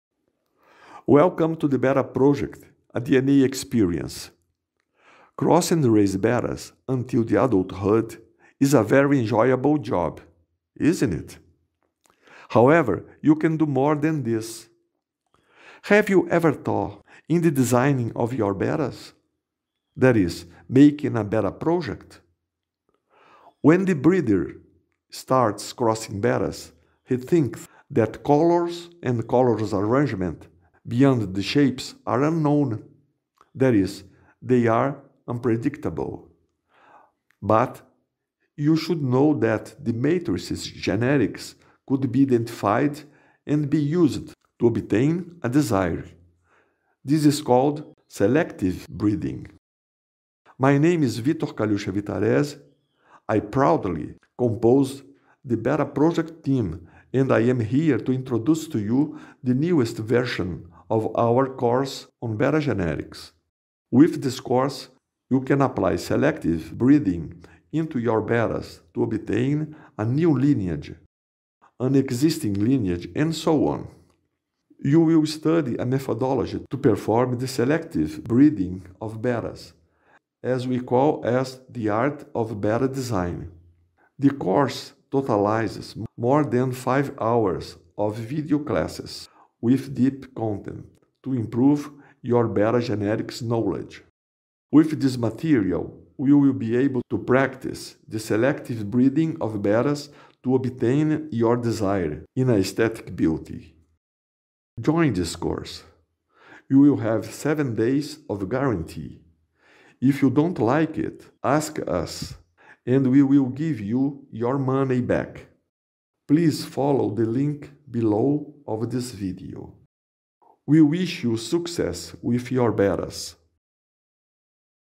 Course on Betta Genetics 2.0: choose matrices for practical application in selective breeding
Module 1: Basic concepts about genetics

We will study basic concepts about genetics to understand what we will cover in the following modules. Concepts such as gene, DNA, genetic trait, chromosome, gametes, locus, allele genes, genotype, phenotype, homozygosity, heterozygosity and other concepts will be addressed.

Module 2: The iridescent layer

We will study how color processes occur in bettas, which biochemical elements are needed. We will also discuss the theoretical model of colors in bettas. We will address all existing traits today about iridescent colors.

Module 3: The black layer

What are cambodia blond bettas, differences in the different actions of these genes in the phenotypes of iridescent and/or pigmented bettas. We will study the size of the melanophores that result in the black lace and melano phenotypes. We will understand why you get large percentages of multicolored fish when working with black bettas. Necessary care in selecting of breeders. Study of superblacks through different types of melanophores. We will also learn about the marble trait, its effect on litters and the difficulty of eliminating it.

Module 4: The red and yellow layers

Know the extended and red loss traits and their behavior in the litters, control of the non-red trait, such as yellow, pineapple, chocolate, pink and orange phenotypes. A discussion of non-red phenotypes will follow. Through discussion and information, we will study our genetic modeling for reds and non-reds, bringing out the types of red pigmentation (cherry and blood pigments) and their interaction exhibited through Punnet Squares, as well as practical considerations of breeder management. We will study the Vf (variegated fin) gene – the butterfly case, technical details that must be observed in butterflies, the performance of the background layer (or yellow layer).

Module 5: Fins in Bettas

We will study the bettas fins with various points of view: structures, types of formats, ramifications, quality of the caudal peduncle and number of lobes of the caudal fins. Understand how the balance between the size of the body and fins of the Betta fish should be. Know which are the genes that involve the number of lobes of the caudal fins. Learn about ventralization in bettas, as well as technical details that can reveal the presence of the double tail (DT) trait in a single tail (ST) phenotype. Types of fin edges, technical details to be observed in these phenotypes regarding the presence of the crown tail trait and its genetics, feather tail and rose tail phenotypes and technical details to be observed. In the end, we will study more about the existence of a certain type of fins, such as bettas dumbos, big ears and big caudal plakat.

Module 6: Masks in Bettas

We will deepen the study on betta masks, defining what they are, what their types are, detailing the current modeling and proposing a new, more comprehensive model for the types of masks. Study of several cases of mating bettas with different masks, technical details that must be observed in the phenotype regarding the presence of a type of partial or total mask.

Module 7: The giant bettas

We will study what giant bettas are, what crosses result in a giant betta, where their natural habitat is, our research and experiences with giant bettas. We will address the current model for studying giant bettas and we will study our proposition of a new model applied to giant bettas.

Module 8: The art of designing bettas

We will make a detailed analysis of a practical case, where it is possible to predict the occurrences of possible phenotypes that will appear in the litter, their theoretical percentages of occurrence, visual observation of the phenotypes of the breeders, information provided by the breeders of our matrices and the concepts studied throughout the course. We will develop in depth the betta black salamander.